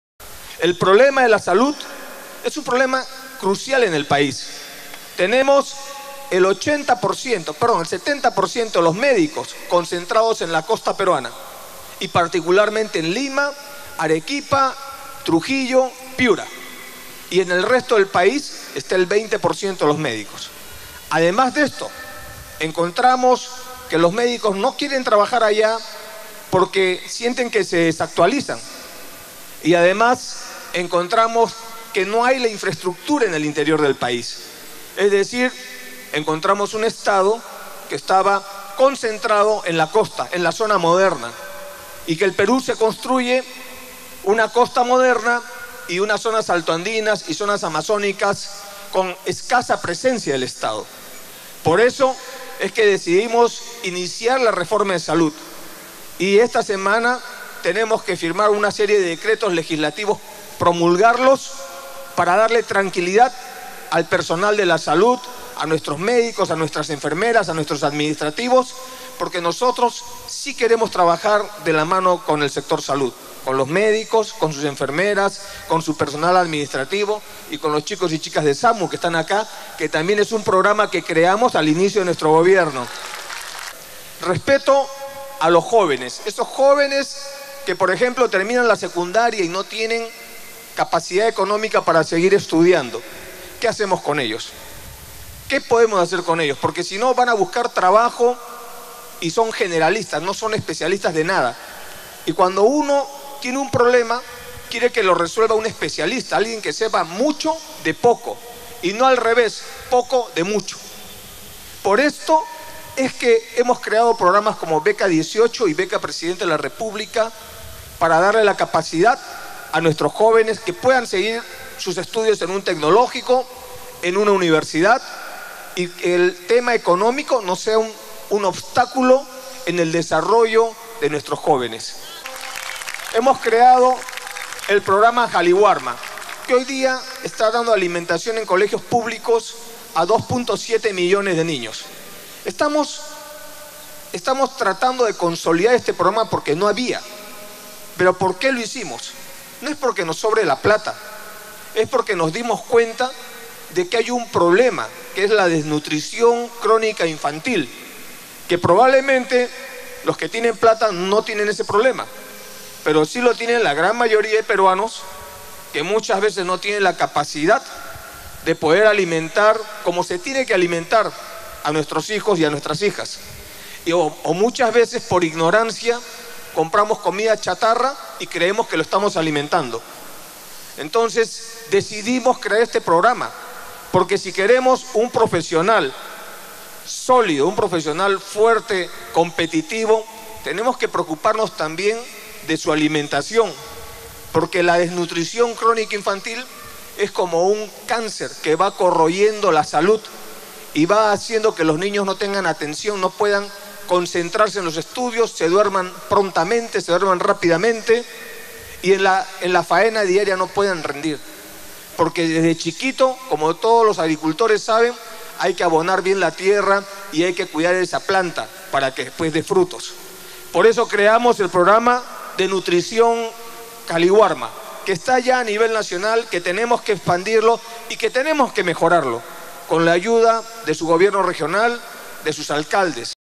Pdte. Humala declara acerca de los temas Salud y Educación en su visita a Huacho
El jefe del Estado anunció desde Huacho, donde inauguró el primer Centro de Atención Integral del Paciente Renal, que se mejorará de manera significativa la oferta pública de servicios de salud en el país, también ratificó el compromiso de su gobierno con la reforma del Estado y con los cambios y programas que se vienen impulsando en el area de la educación.